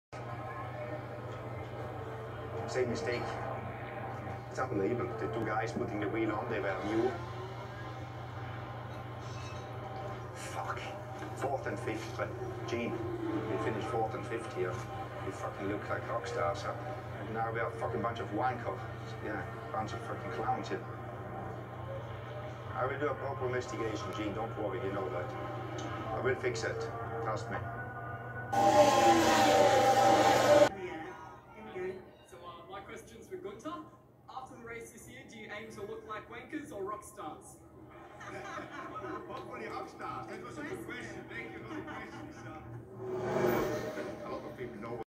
Formula 1 Grand Prix Funniest question and answer
Haas team principle Guenther Steiner lends some humor to a bad situation.